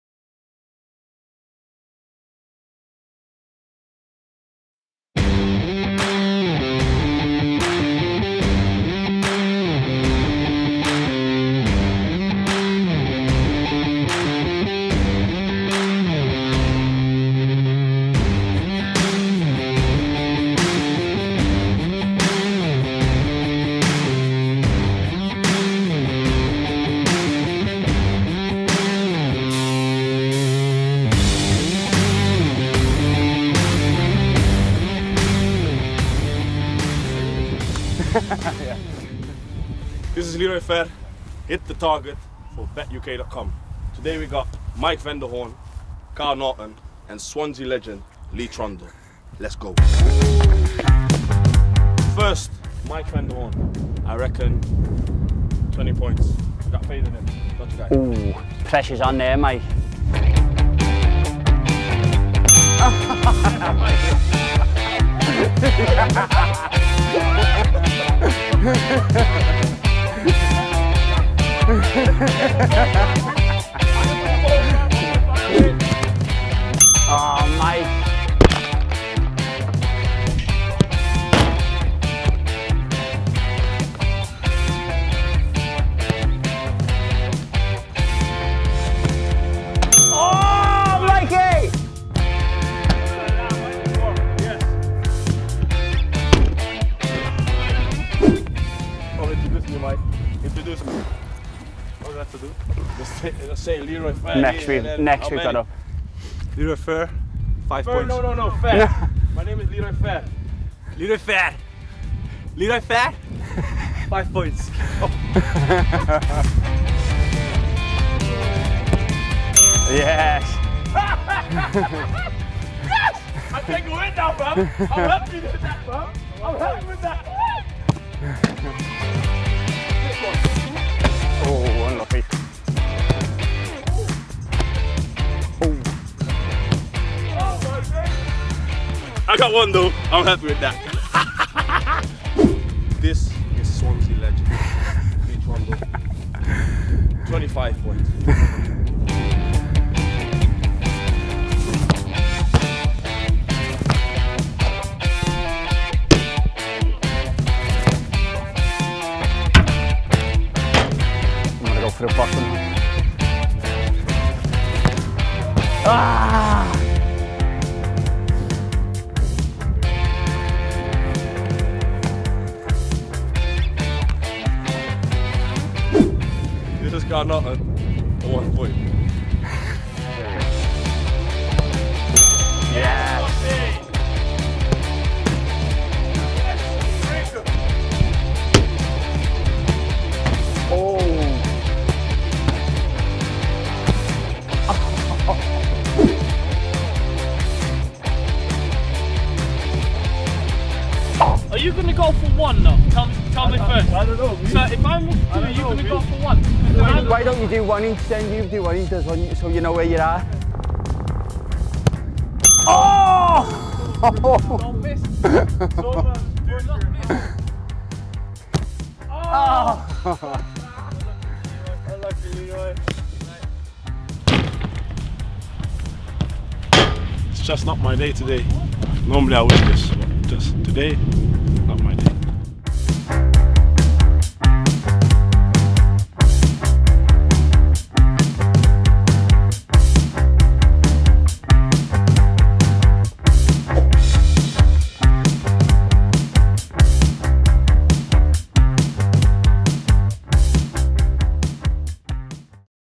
🎯HIT THE TARGET | Shooting Challenge
. . . Swans stars Leroy Fer, Kyle Naughton and Mike van der Hoorn  were joined by legend Lee Trundle in a shooting accuracy challenge. 10 points for the top corners, 1 point for the bottom, and 5 points for the middle. See who emerged the winner!